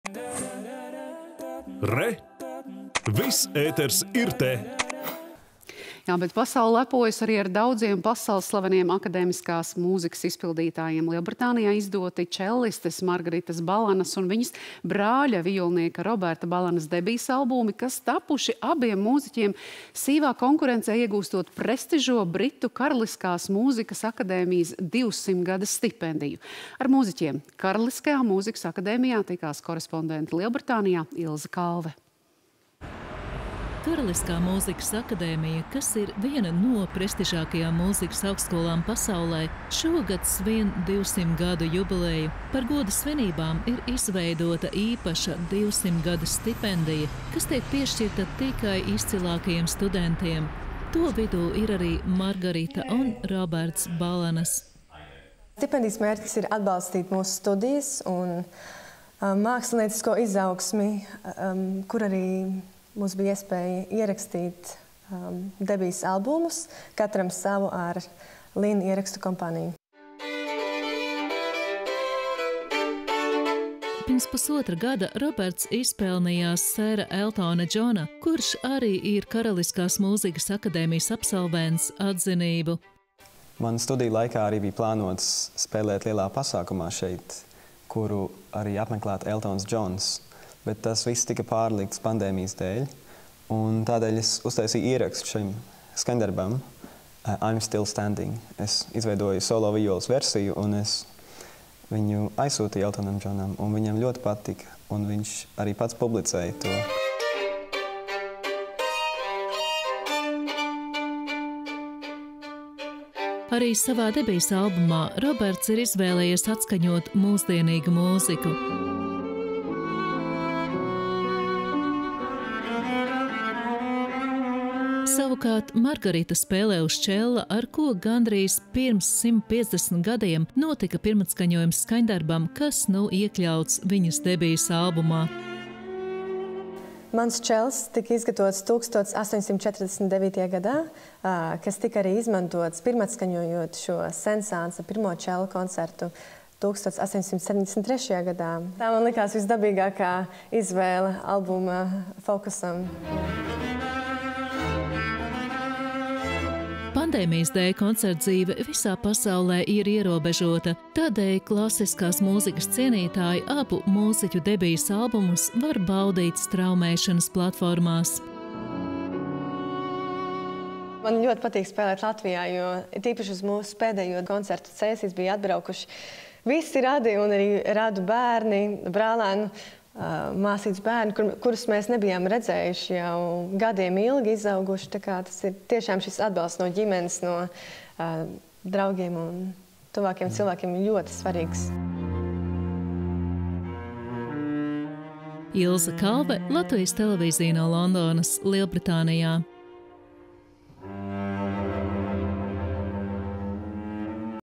Karaliskā Mūzikas akadēmija novērtē latviešu mūziķu paveikto
Latvija var lepoties ne ar vienu vien pasaulslavenu akadēmiskās mūzikas izpildītāju un sevi pārliecinoši piesaka arvien jauni vārdi. Nupat kā Lielbritānijā izdoti čellistes Margaritas Balanas un viņas brāļa vijolenieka Roberta Balanas debijas albumi, kas tapuši, abiem mūziķiem sīvā konkurencē iegūstot prestižo britu Karaliskās Mūzikas Akadēmijas „Divsimtgades stipendiju”. Mūziķus satikt uz Karalisko Mūzikas akadēmiju devās mūsu korespondente Lielbritānijā Ilze Kalve.